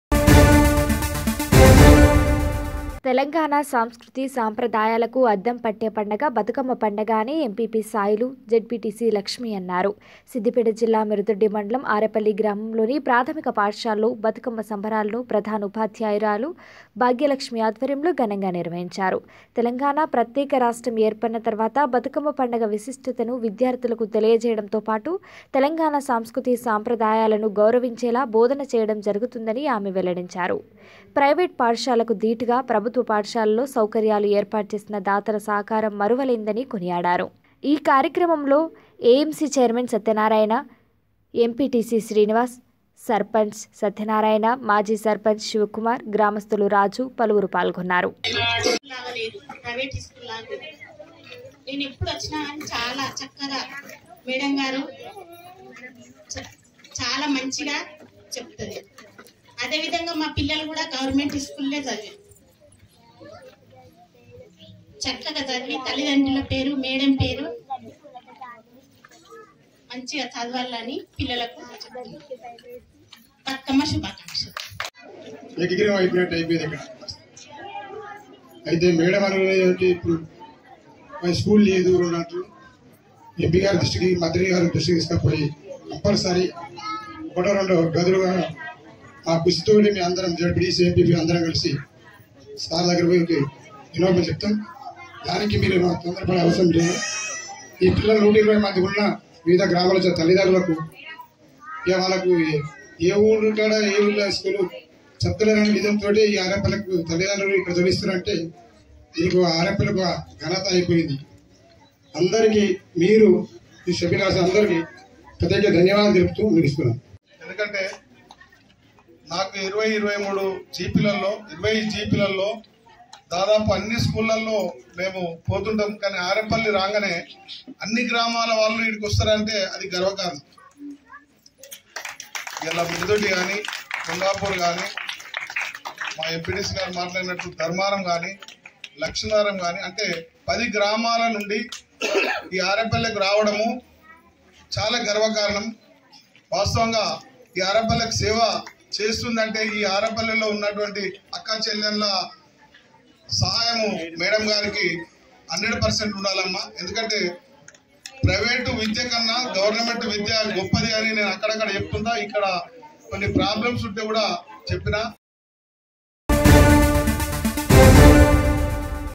తెలంగాణ సంస్కృతి సాంప్రదాయాలకు అద్దం పట్టే పండుగ బతుకమ్మ పండుగ అని ఎంపీపీ సాయిలు జడ్పిటిసి లక్ష్మి
తెలంగాణ సంస్కృతి సాంప్రదాయాలకు అద్దం పట్టే పండుగ బతుకమ్మ పండుగ అని ఎంపీపీ సాయిలు జడ్పిటిసి లక్ష్మి అన్నారు. సిద్దిపేట జిల్లా మిరుదొడ్డి మండలం ఆరెపల్లె గ్రామంలోని ప్రాథమిక పాఠశాలలో బతుకమ్మ సంబరాలను ప్రధానోపాధ్యాయురాలు భాగ్యలక్ష్మి ఆధ్వర్యంలో ఘనంగా నిర్వహించారు. తెలంగాణ ప్రత్యేక రాష్ట్రం ఏర్పడిన తర్వాత బతుకమ్మ పండుగ విశిష్టతను విద్యార్థులకు తెలియజేయడంతో పాటు తెలంగాణ సంస్కృతి సాంప్రదాయాలను గౌరవించేలా బోధన చేయడం జరుగుతుందని ఆమె వెల్లడించారు. ప్రైవేటు పాఠశాలలకు దీటుగా ప్రభుత్వ పాఠశాలలో సౌకర్యాలు ఏర్పాటు చేసిన దాతల సహకారం మరువలేనిదని కొనియాడారు. అనంతరం పాఠశాల అభ్యున్నతి కోసం సహకరించిన దాతలను ప్రజా ప్రతినిధులచే ఘనంగా సన్మానించారు. పాఠశాలలో మెరుగైన విద్యా బోధనకు కృషి చేస్తున్న ప్రధానోపాధ్యాయులు భాగ్యలక్ష్మిని ఎంపీపీ సాయిలు, జడ్పిటిసి లక్ష్మి అభినందించారు. ఈ కార్యక్రమంలో ఏఎంసి చైర్మన్ సత్యనారాయణ, ఎంపీటీసీ శ్రీనివాస్, సర్పంచ్ సత్యనారాయణ, మాజీ సర్పంచ్ శివకుమార్, గ్రామస్తులు రాజు పలువురు పాల్గొన్నారు.\